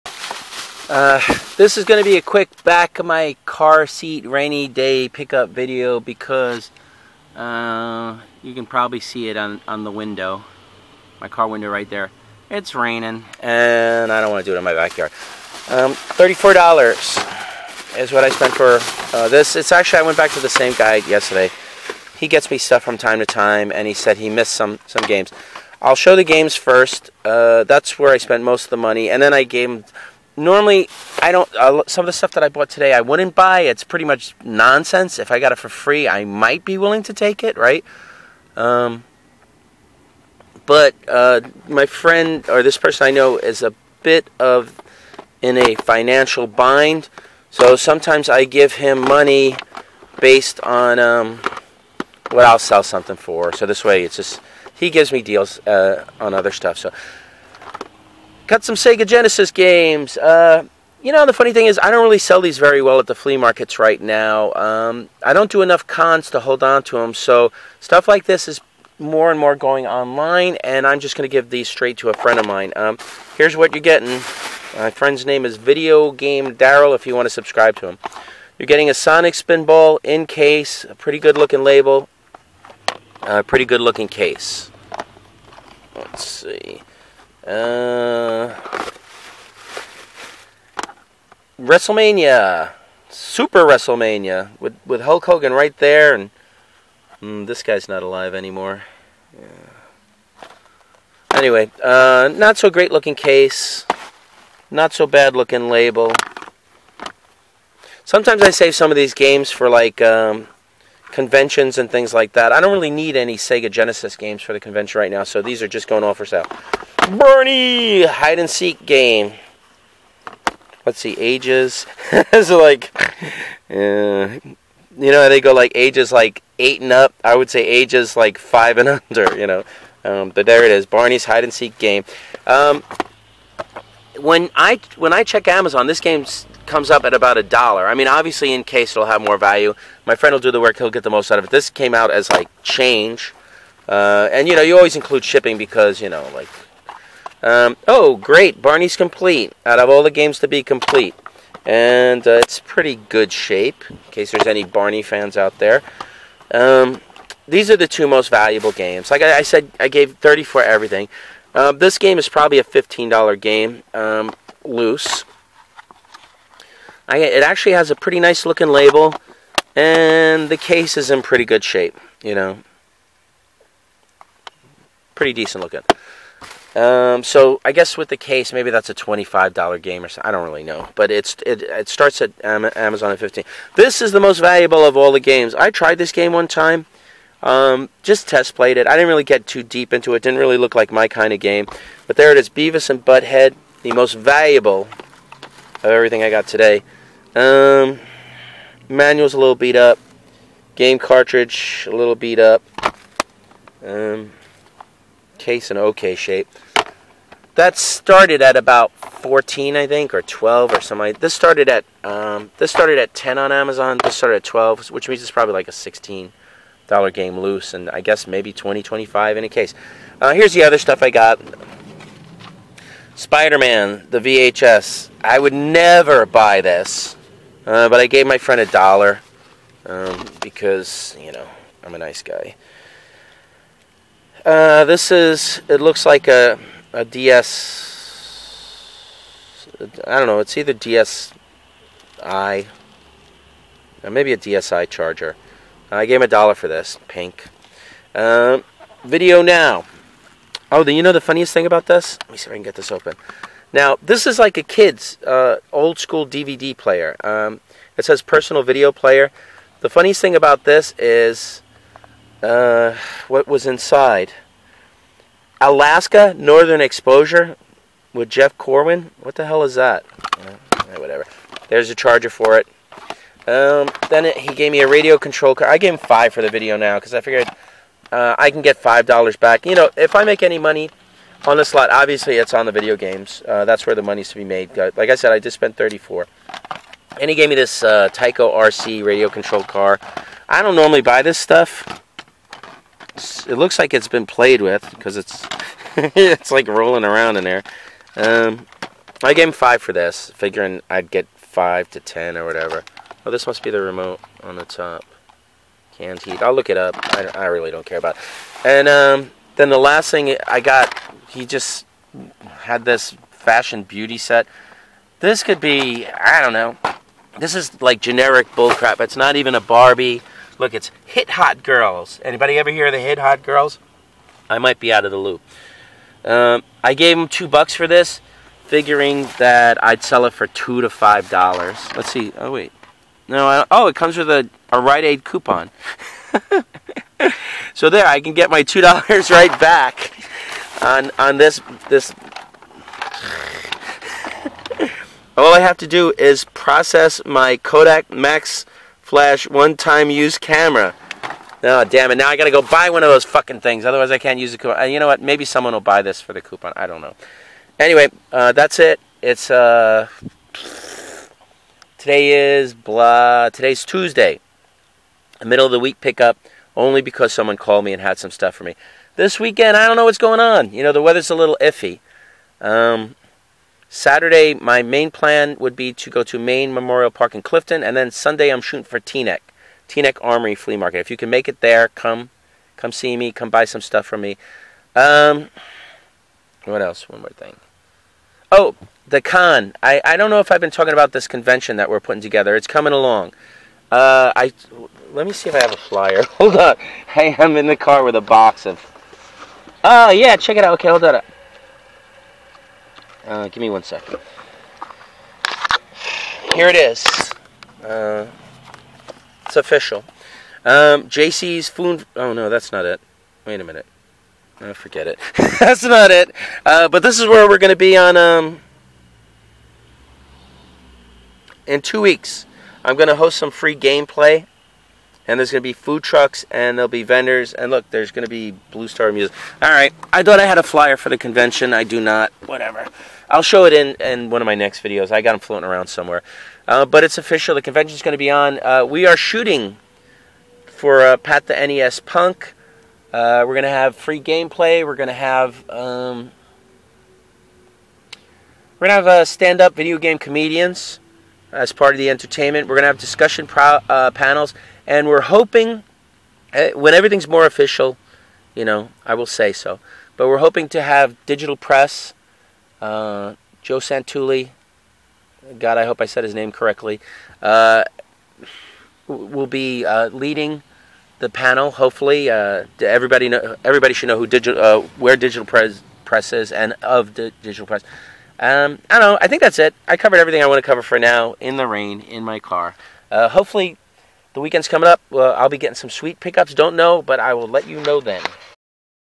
Video Games Sega Genesis Toys. Pick Ups from an Appointment - 3/28/17
Here's some stuff that I got from an appointment today.

7-34